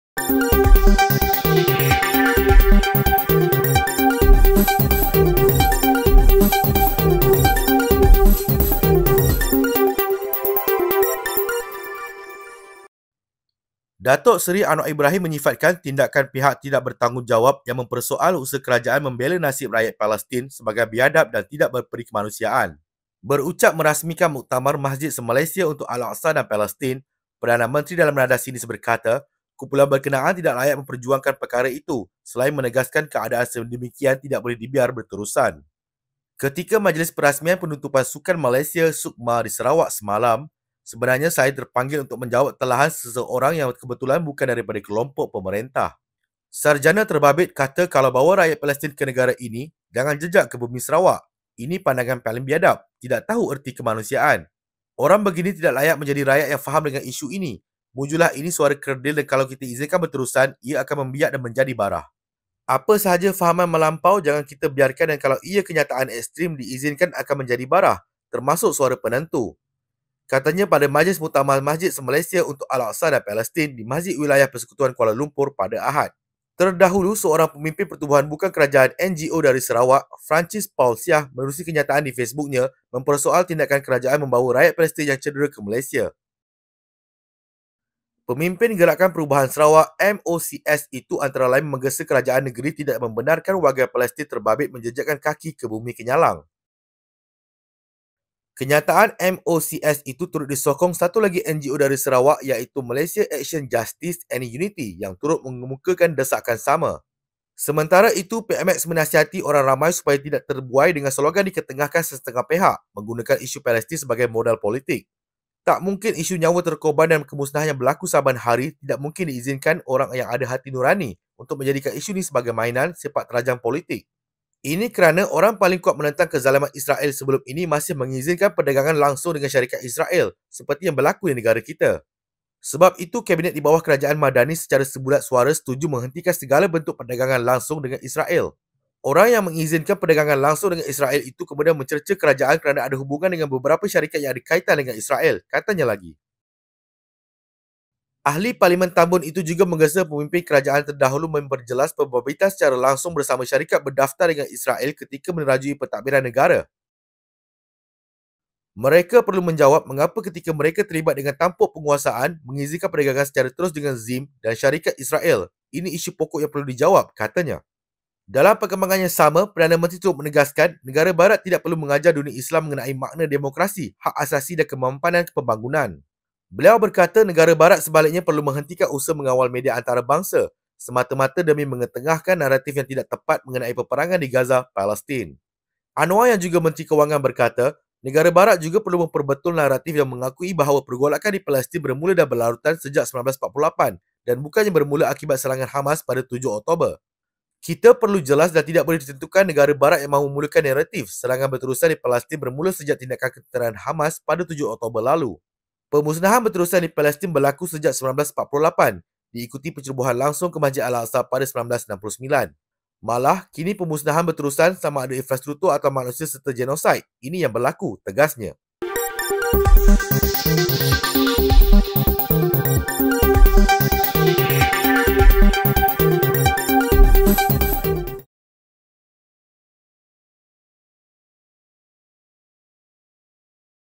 26.08.2024 - MADANI TERKINI | ‘KUMPULAN PERSOAL BANTUAN KERAJAAN KEPADA PALESTIN ADALAH BIADAB’-PMX
Dato’ Seri Anwar Ibrahim menyifatkan tindakan pihak tidak bertanggungjawab yang mempersoal usaha kerajaan membela nasib rakyat Palestin sebagai biadab dan tidak berperikemanusiaan.